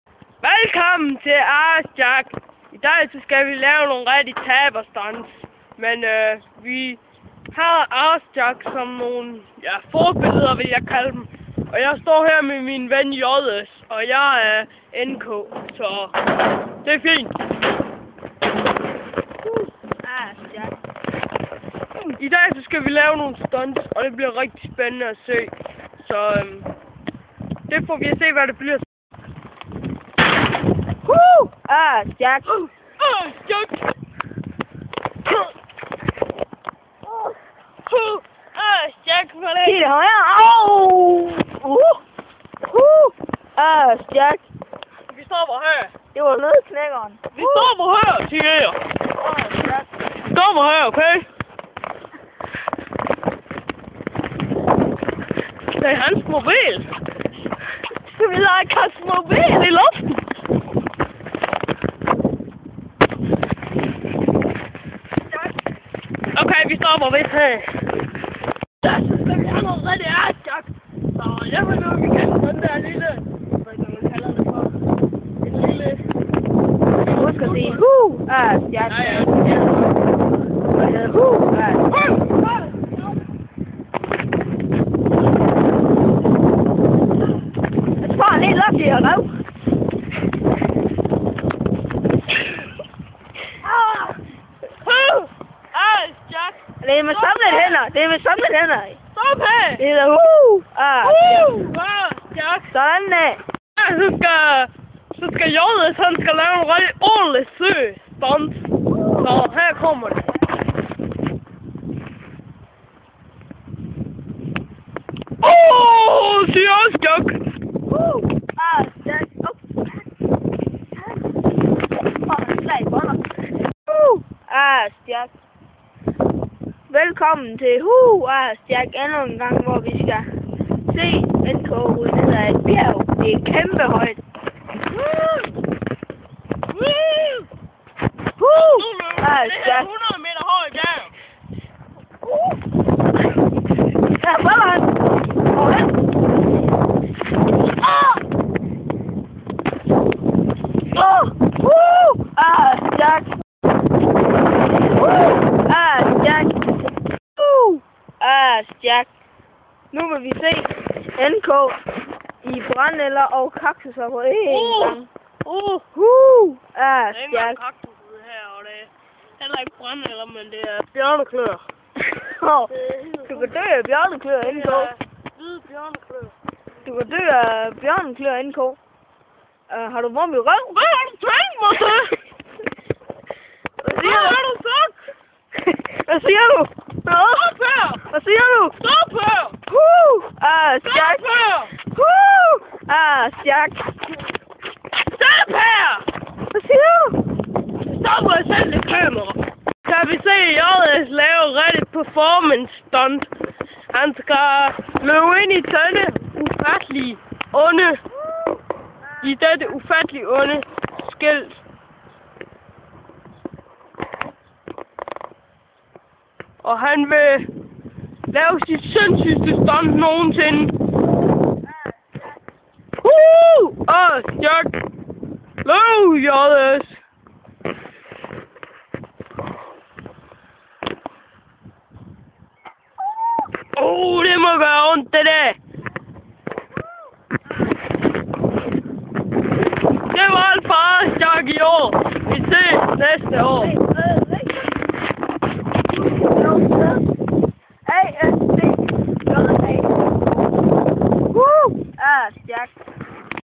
AssJack (Special)
This movie is about two dumb assholes trying to look like AssJack (REAL) /Jackass... Please we know this is terrible, you do not have to rite it..!:D